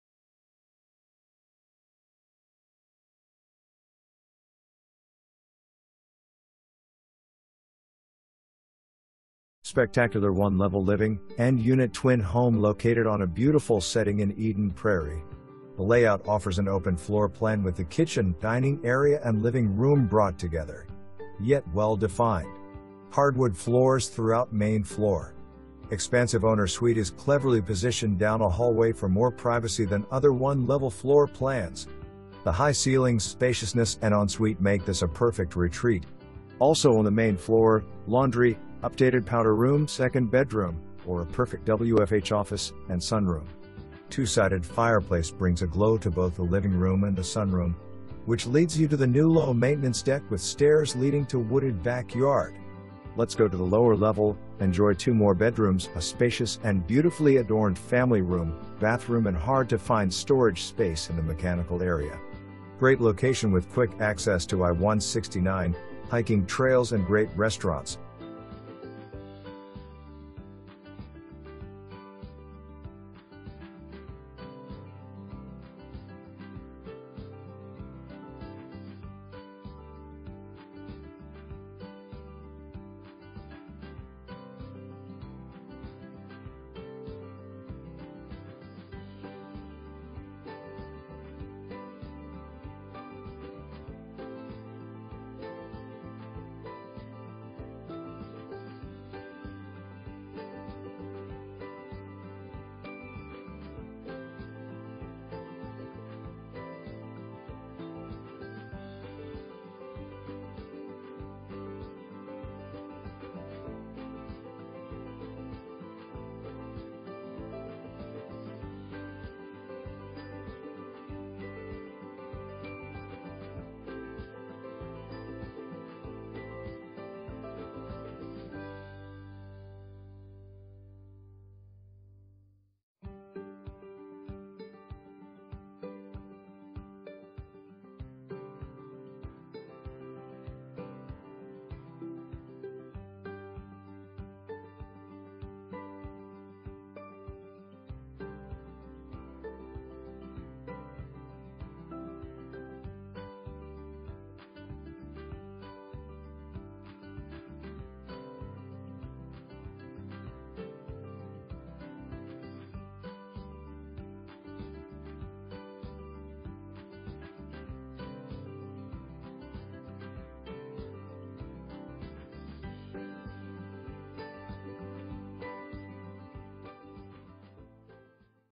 10038 Lee Drive
Spectacular one level living - end unit twin home located on a beautiful setting in Eden Prairie. The layout offers an open floor plan with the kitchen, dining area and living room brought together, yet well defined. Hardwood floors throughout main floor. Expansive owner's suite is cleverly positioned down a hallway for more privacy than other one level floor plans. The high ceilings, spaciousness and ensuite make this a perfect retreat. Also on the main floor - laundry, updated powder room, 2nd bedroom (or a perfect WFH office), and sunroom. 2-sided fireplace brings a glow to both the living room and the sunroom, which leads you to the new low maintenance deck with stairs leading to wooded back yard. Let's go to the lower level: enjoy two more bedrooms, a spacious and beautifully adorned family room, bathroom and hard to find storage space in the mechanical area. Great location next to Pioneer Trail allows for privacy from other units. Assn dues for general liability only.

1 full bath, 1 half bath, 1 three-quarter bath

Laura Lee Berger
Coldwell Banker Realty